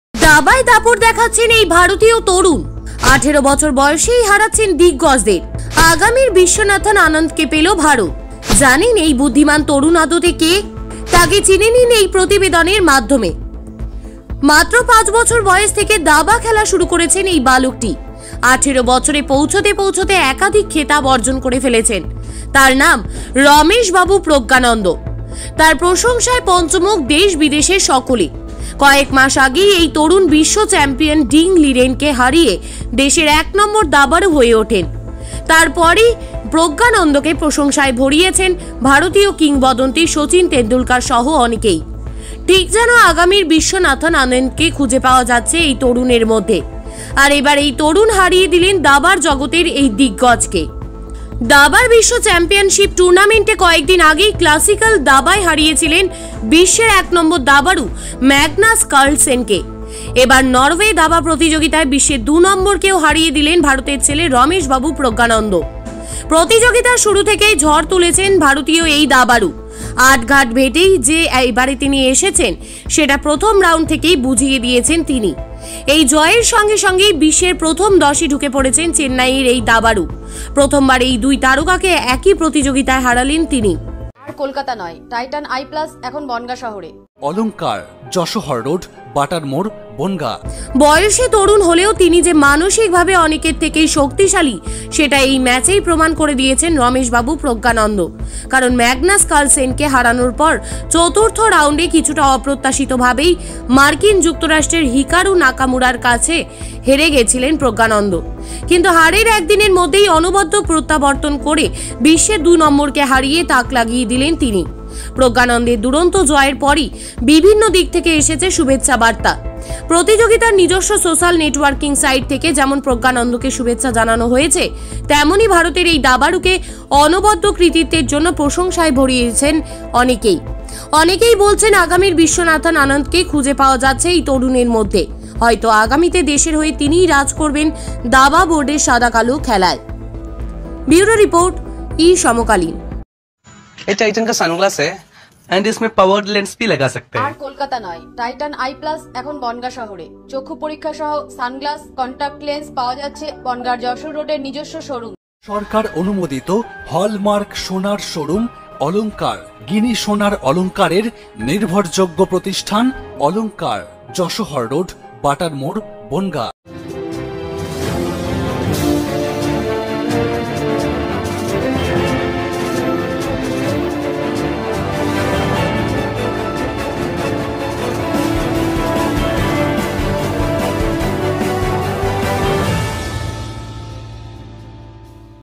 তরুণ ভারতীয় দাবাড়ুর দাপট‌ - The power of young Indian chess players
তরুণ ভারতীয় দাবাড়ুর দাপট‌‌
The power of young Indian chess players

মাত্র ৫ বছর বয়স থেকে দাবা খেলা শুরু করেছেন এই বালকটি। ১৮-বছরে পৌঁছতে পৌঁছতে একাধিক খেতাব অর্জন করে ফেলেছেন। তাঁর নাম রমেশ বাবু প্রজ্ঞানন্দ। তাঁর প্রশংসায় পঞ্চমুখ দেশ বিদেশের সকলে। কয়েকমাস আগেই এই তরুণ বিশ্ব চ্যাম্পিয়ন ডিং লিরেনকে হারিয়ে দেশের এক নম্বর দাবাড়ু হয়ে ওঠেন। তারপরই প্রজ্ঞানন্দকে প্রশংসায় ভরিয়েছেন ভারতীয় কিংবদন্তি সচিন তেন্ডুলকর সহ অনেকেই। ঠিক যেন আগামীর বিশ্বনাথন আনন্দকে খুঁজে পাওয়া যাচ্ছে এই তরুণের মধ্যে। আর এবার এই তরুণ হারিয়ে দিলেন দাবার জগতের এই দিগগজকে।